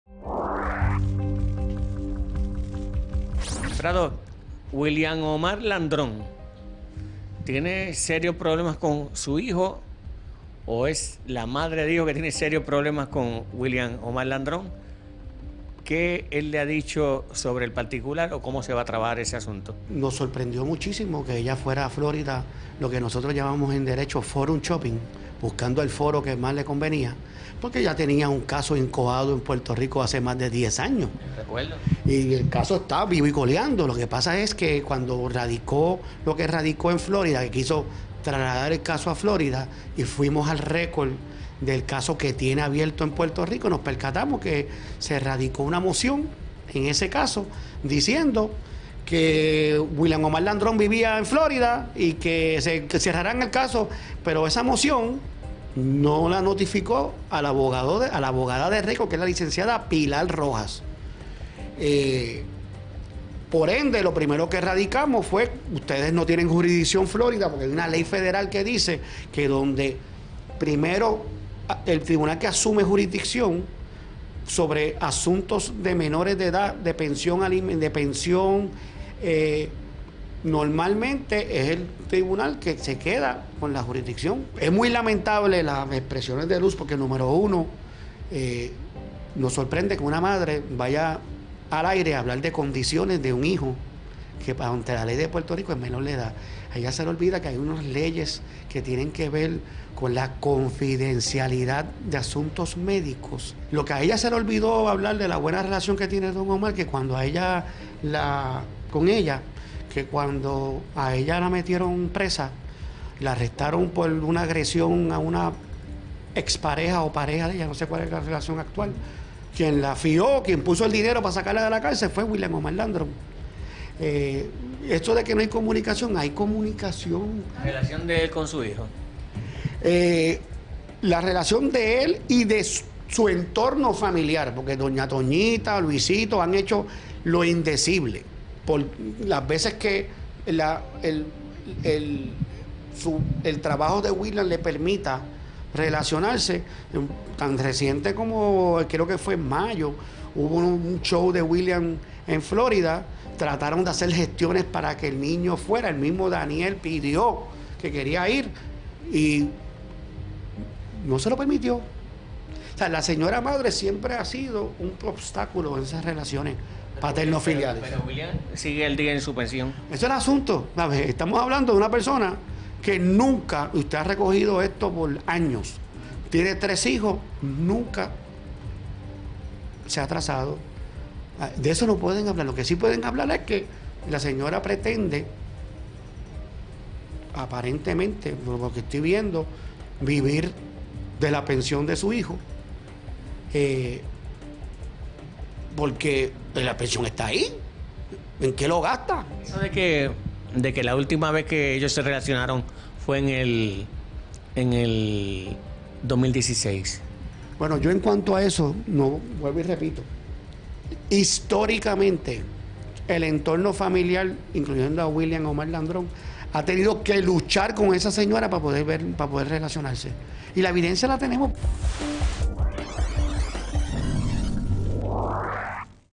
Entrevista al Lcdo. Edwin Prado - 29 octubre 2019
Nuestro "Paparazzi" Leo Fernández III entrevistó al Licenciado Edwin Prado encargado de representar al reguetonero Don Omar en el caso de pensión alimentaria y relaciones filiales, este nos reacciona a las expresiones que realizará Luz VIllanueva ex pareja del reguetonero.

Comparte que esta buscando que el caso continue su curso en Puerto Rico, porque es la jurisdicción a la que corresponde, por Don Omar vivir en la Isla. Radicaron una moción para que señalen una fecha. El artista nunca le ha fallado en las pensiones a sus hijos. Dice que alegadamente es ella la que no permite que Don OMar se acerque a su hijo. Ella no permitió que fuera al concierto de Don Omar en la Florida, ni que hay una relación saludable entre ellos.